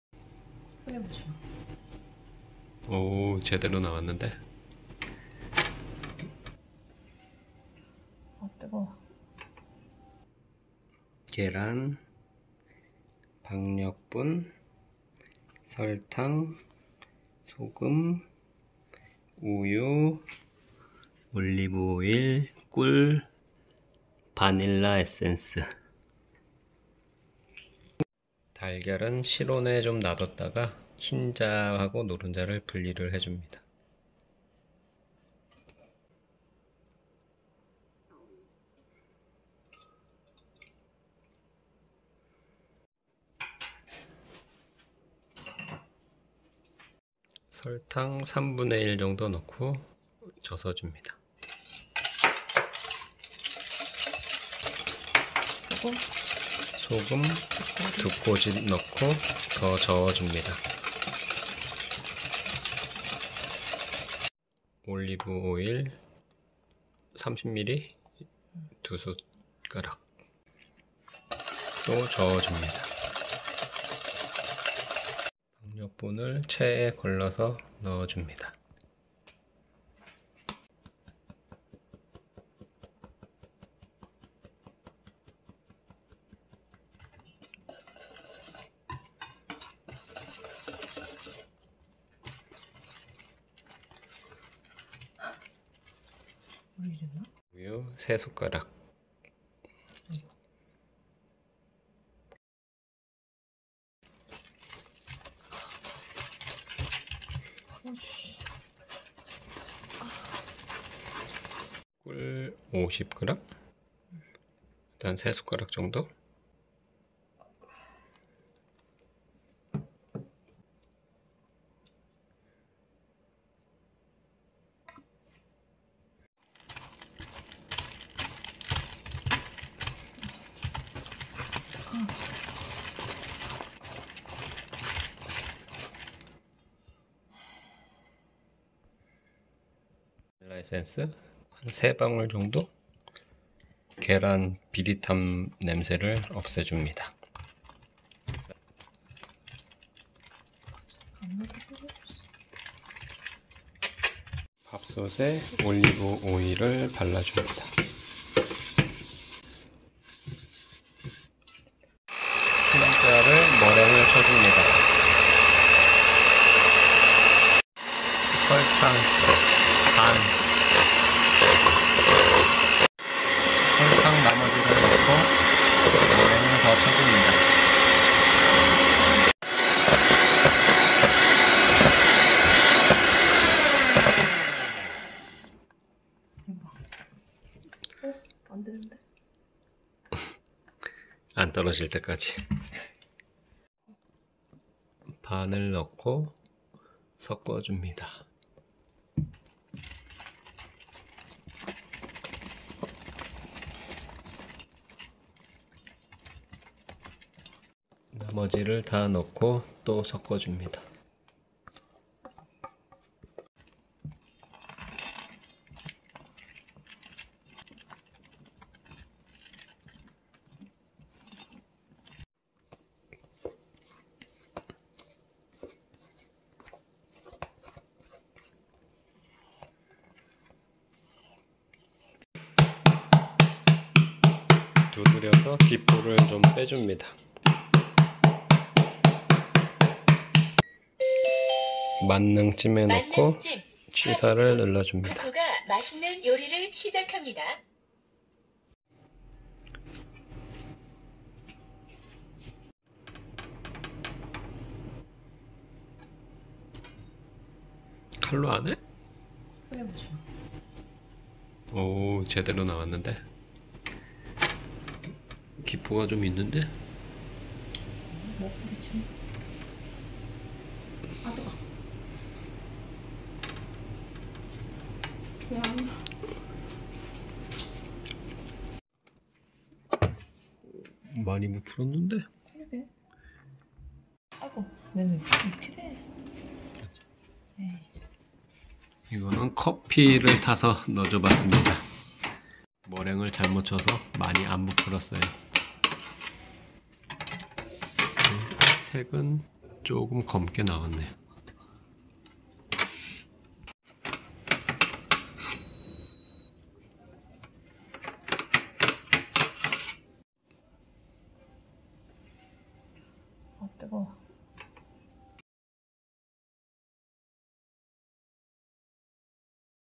전기밥솥으로 간단히 카스테라 만들기
전기밥솥으로 카스테라 만들어 봤습니다.
열심히 섞다보면 카스테라가 완성 되네요.